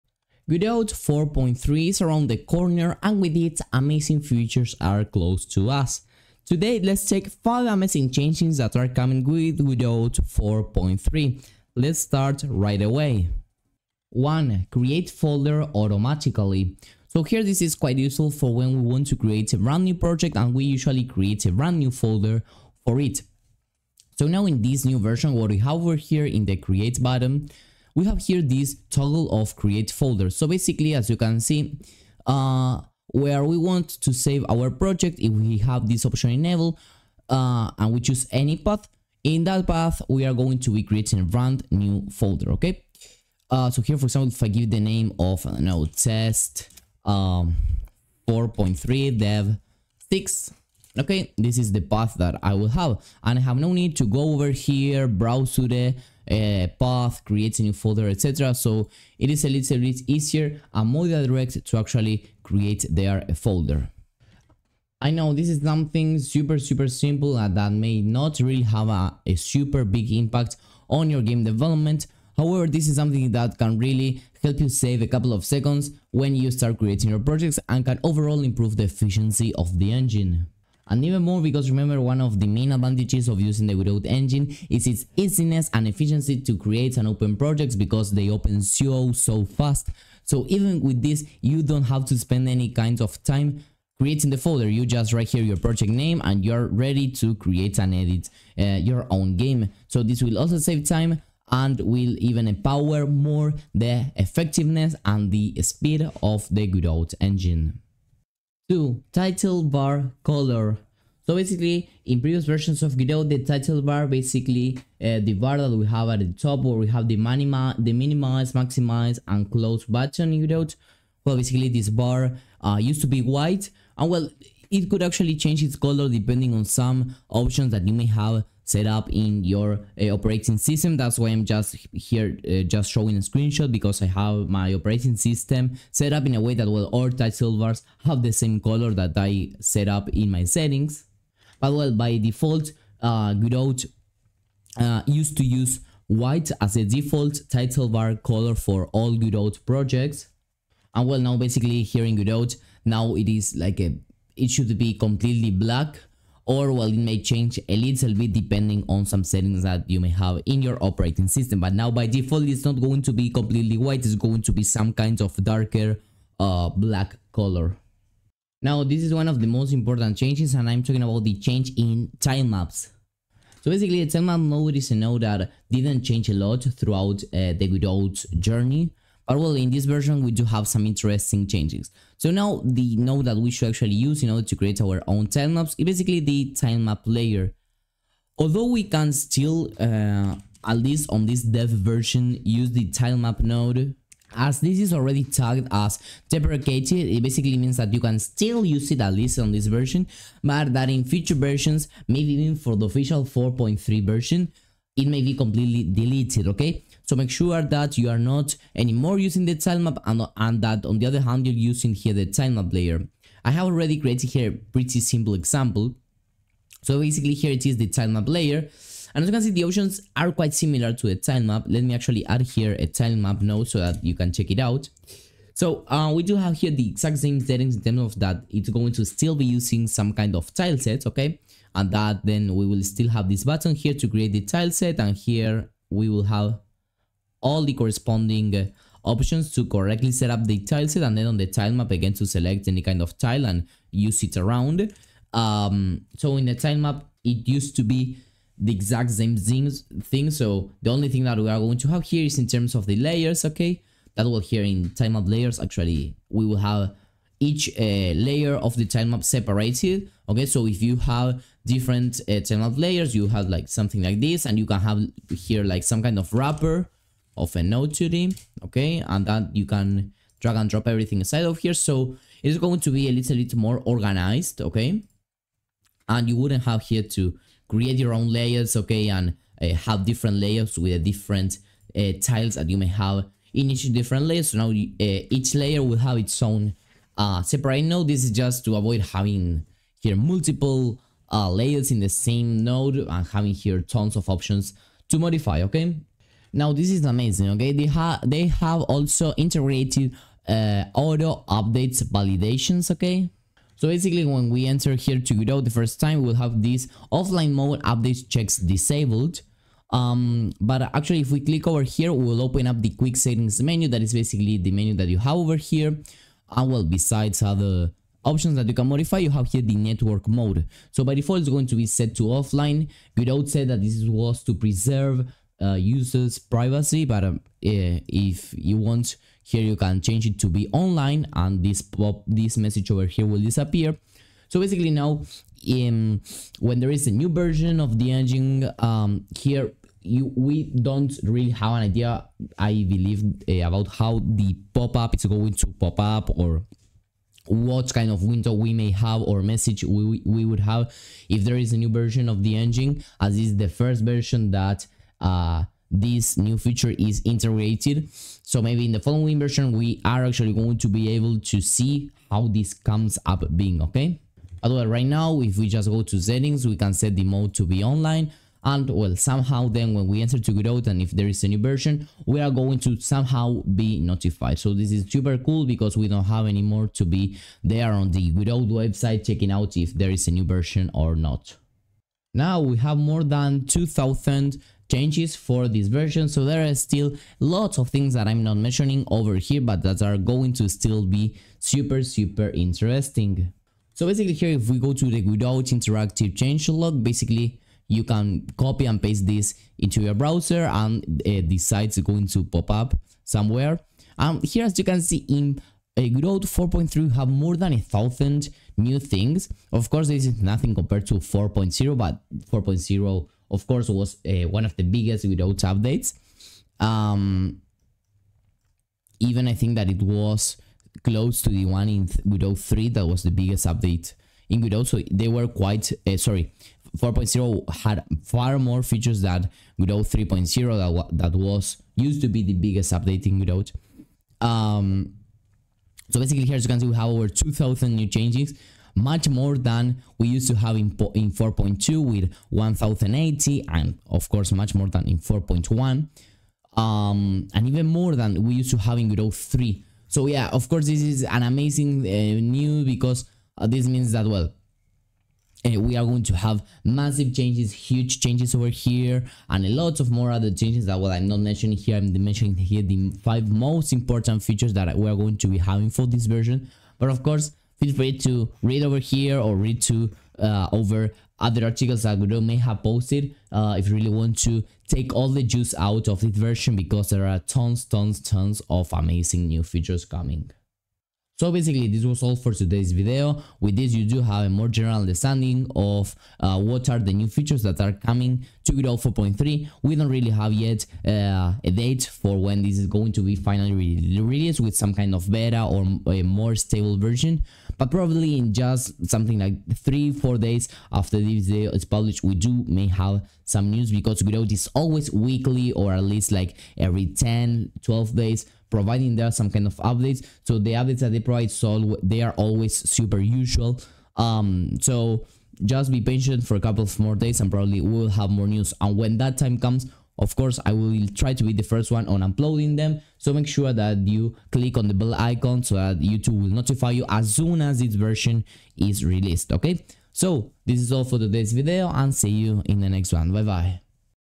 5 Amazing Changes in Godot 4.3
Godot 4.3 is around the corner and with it amazing features are close to us. Today, let’s check 5 amazing changes that are coming with Godot 4.3, let’s start right away